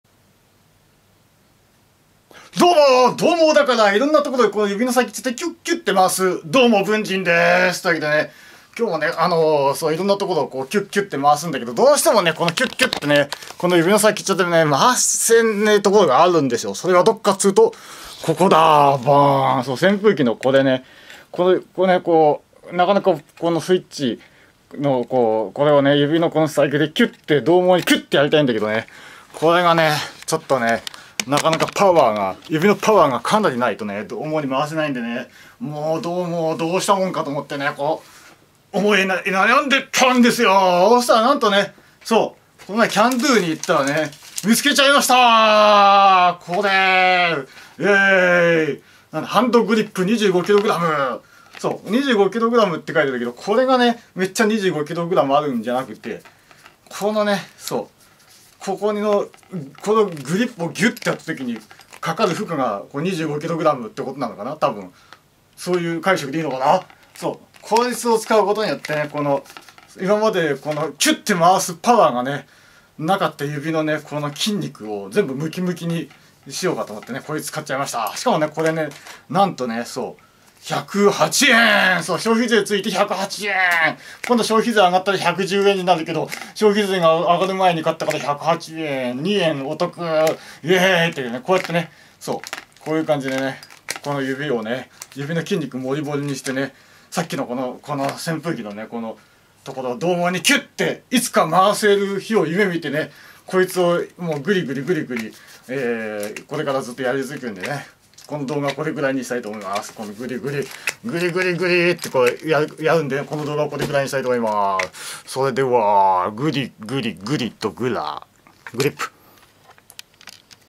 獰猛に指でツマミをキュってするために25kgのハンドグリップを買ったのだ！
BUNZINです。
扇風機のメッチャ細いツマミを獰猛に指でツマミをキュってするために25kgのハンドグリップを買ったのだ！メッチャ指を鍛えてツマミを獰猛にキュってするのだ！